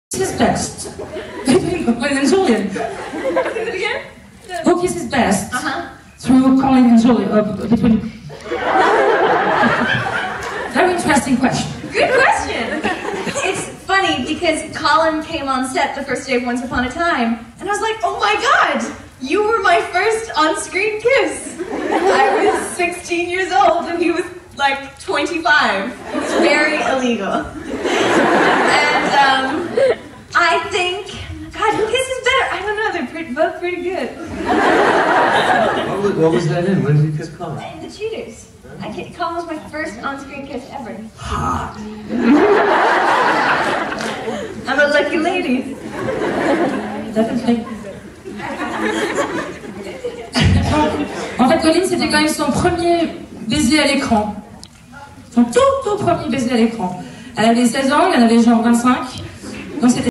Sarah Bolger talks about her first on-screen kiss with Colin O'Donoghue
Fairytales Convention 2013